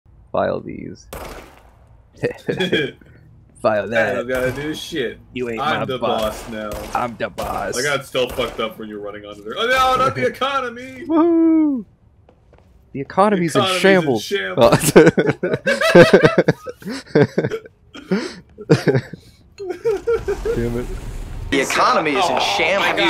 The Economy is in Shambles
Dep and Pega play control and talk about the economy.
Check us out live on our Twitch channels.